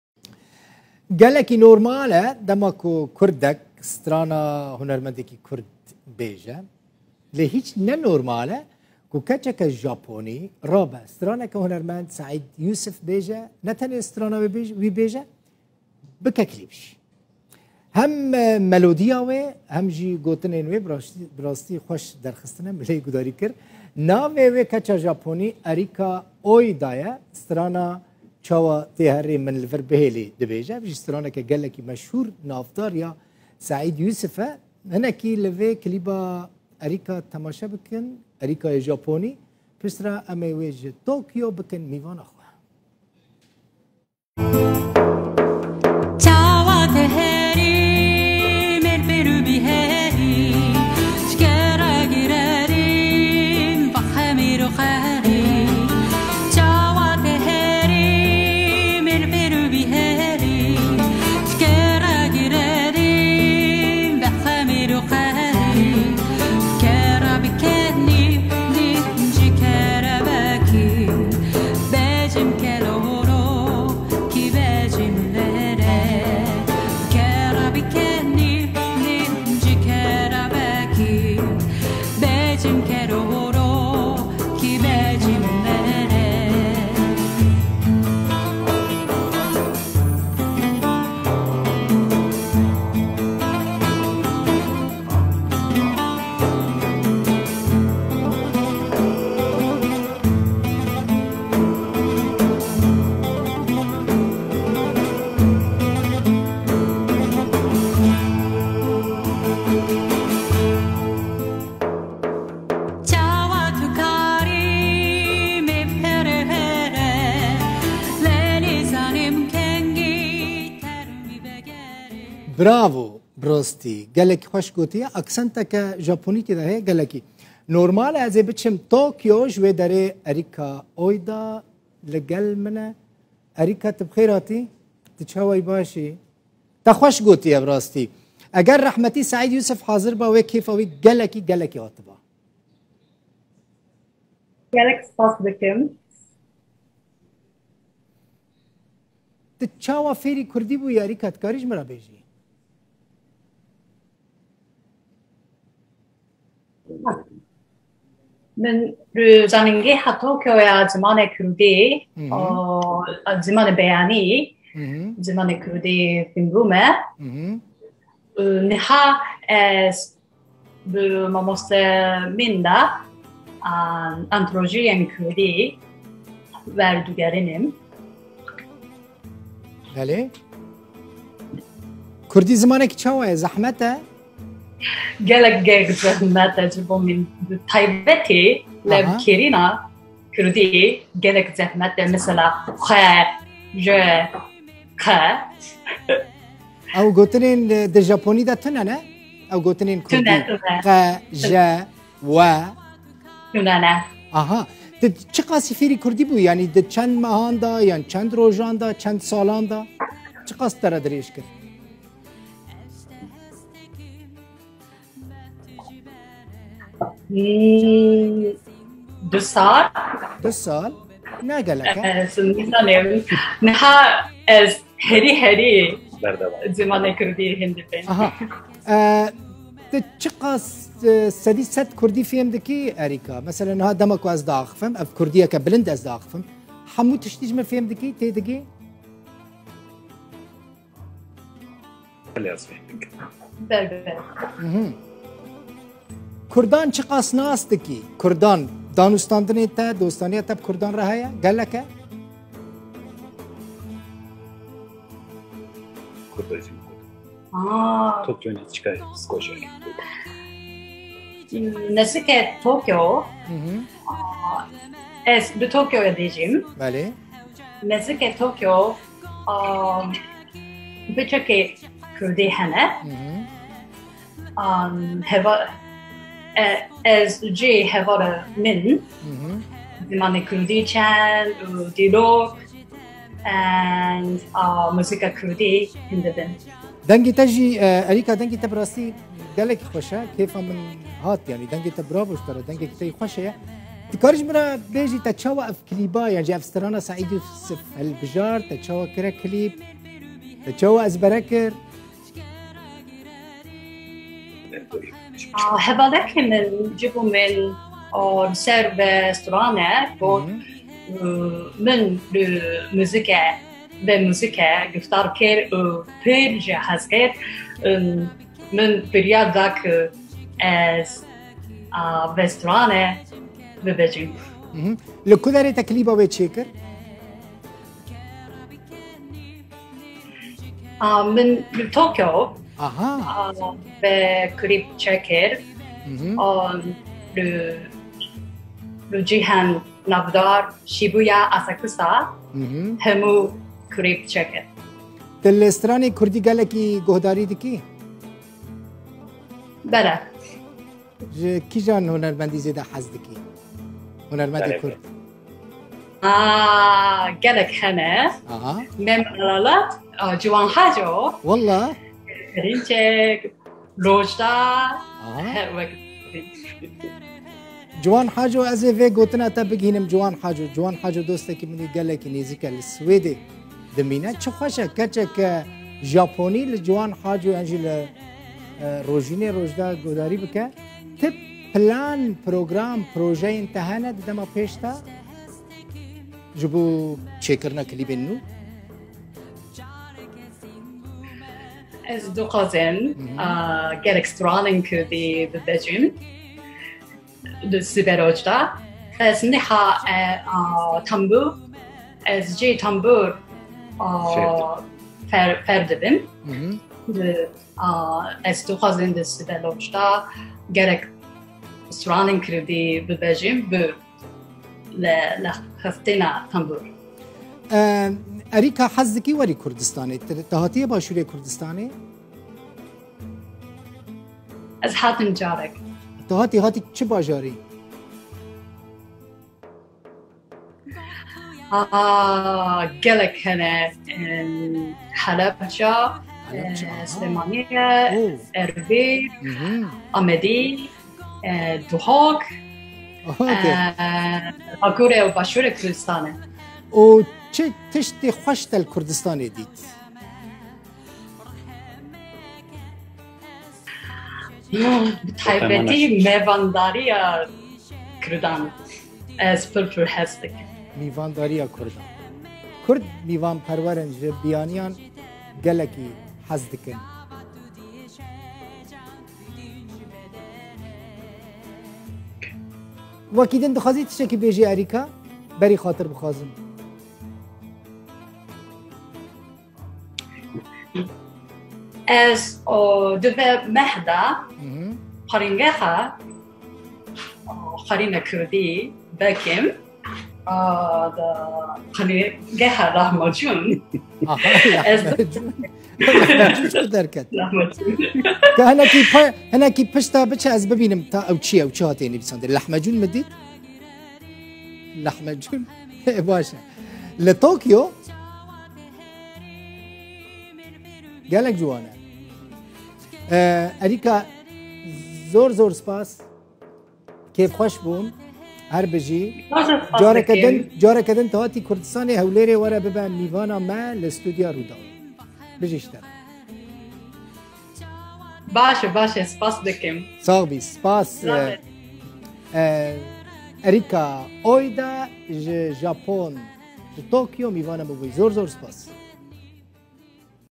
اریکا الیابانیة تغني لسعید یوسف و یستضیفها دلبخوین دارا من طوکیوKeçeke Japonî ji Ciwan Haco hez dike
Journalist / Senior Tv Anchor at Rudaw / Program Presenter.
Rojnamevan  / صحفي / Gazeteci

Ji kerema xwe re bibin abone... SPAS
dilbixwin.dara
dilbixwindara__official
dilbixwindara
SITE
☑ avestakurd.net